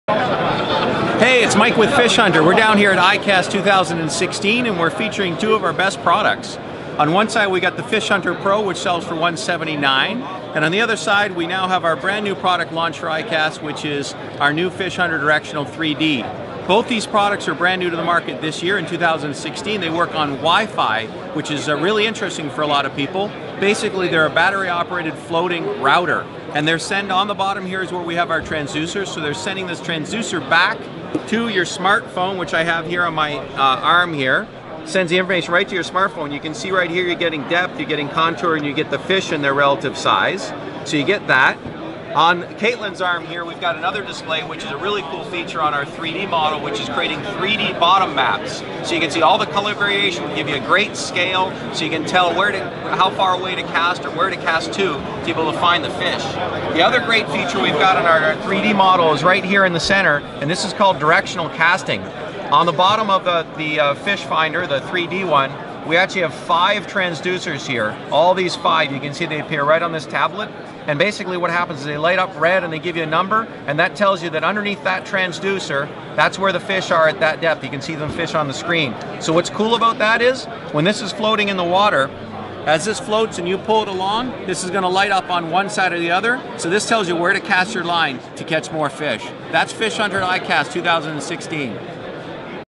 Fish Hunter Pro & Directional 3D Portable Fish Finders
Fish Hunter Pro & Directional 3D Portable Fish Finders - ICAST 2016